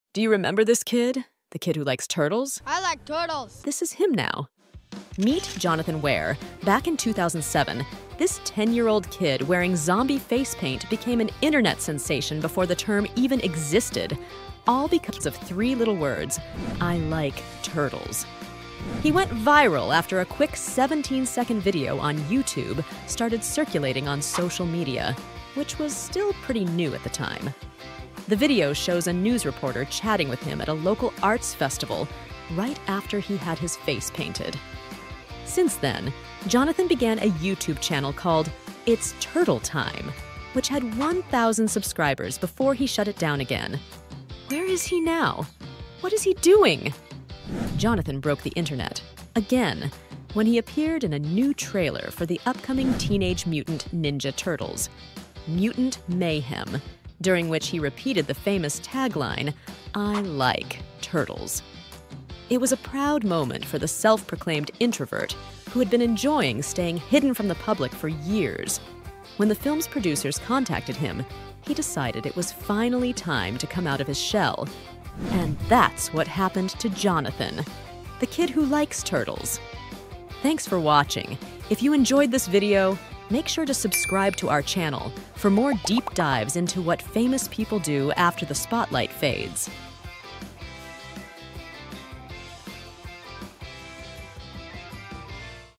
Where Is "I Like Turtles Meme Kid" Now?
Discover What Happened To Jonathan Ware? The Kid Who Likes Turtles

Timestamps
00:00 Who is Jonathan Ware
00:19 How Jonathan Ware Go Viral
00:35 Jonathan Ware Then
00:45 Jonathan Ware Now
01:16 The Reason What Happened to Jonathan Ware